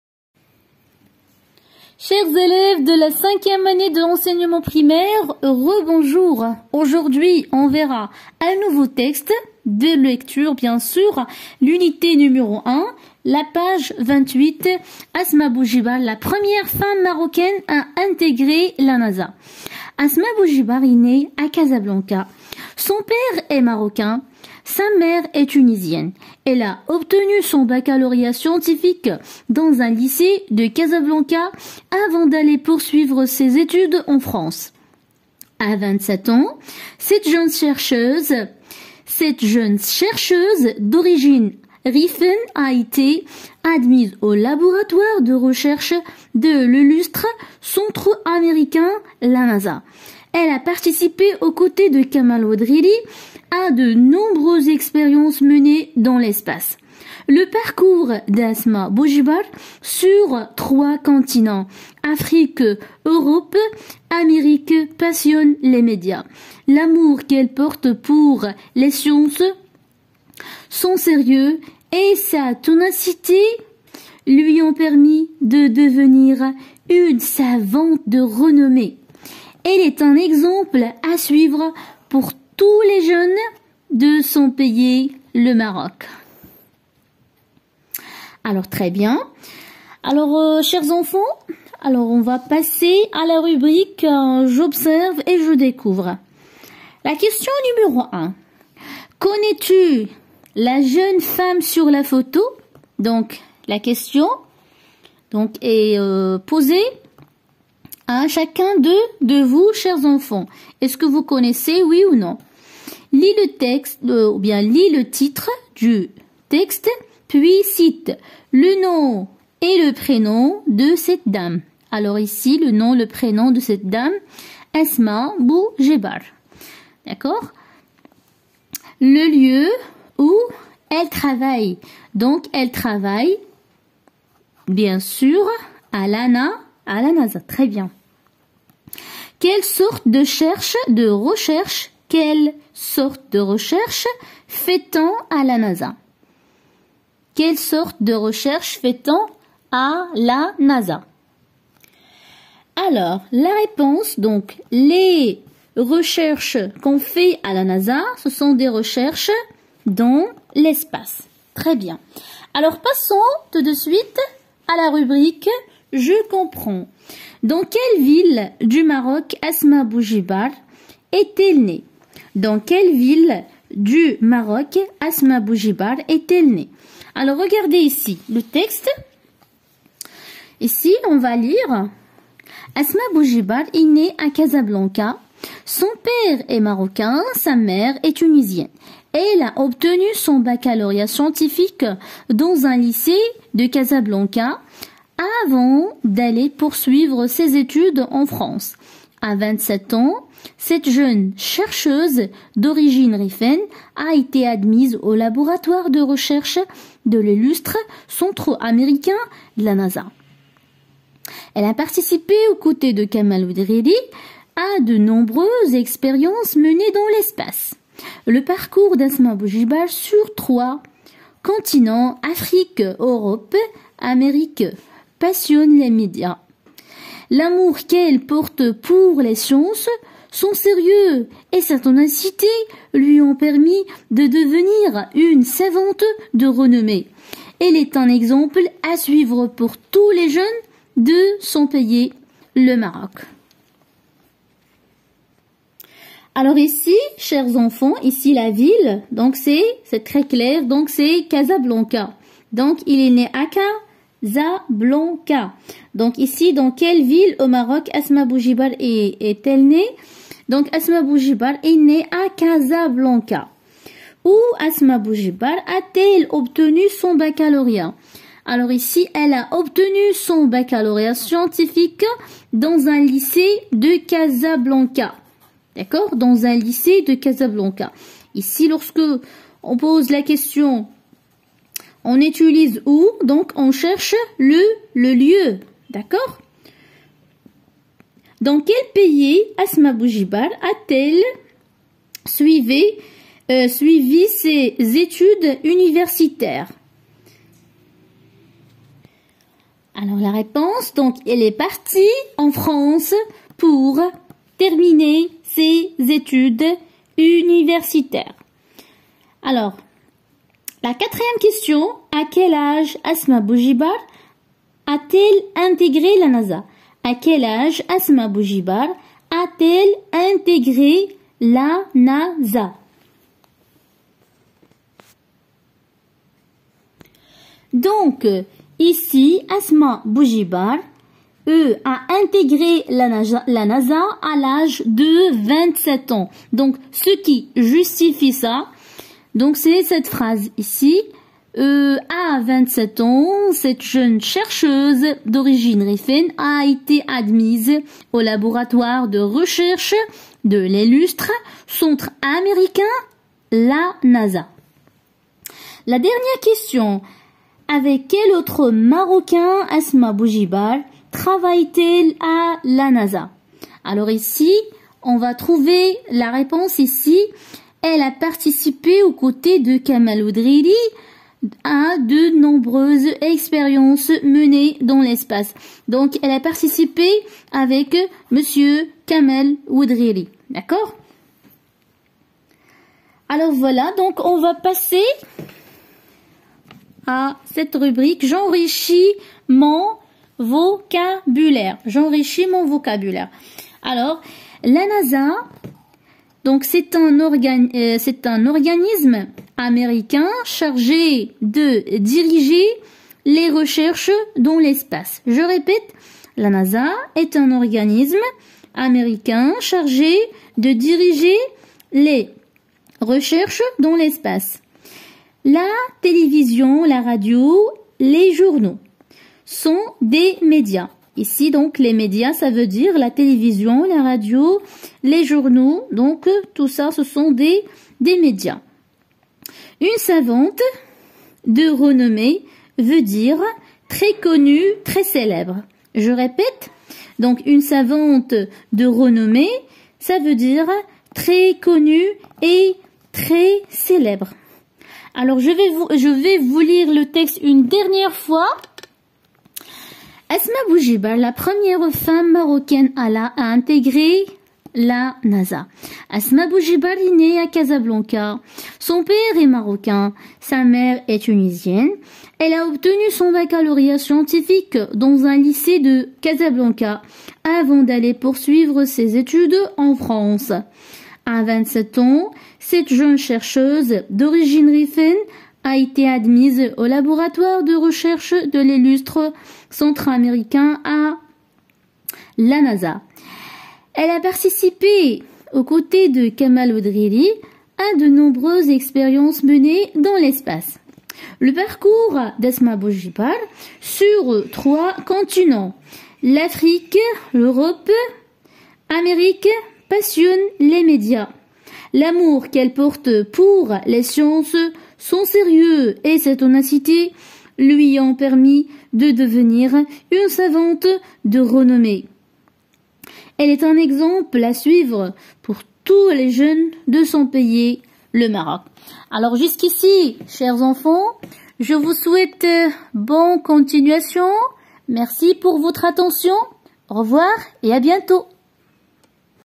Lecture :Asmae BOUJIBAR, la première femme marocaine à intégrer la NASA .P28
Pour communiquer en français
5eme année de l'enseignement primaire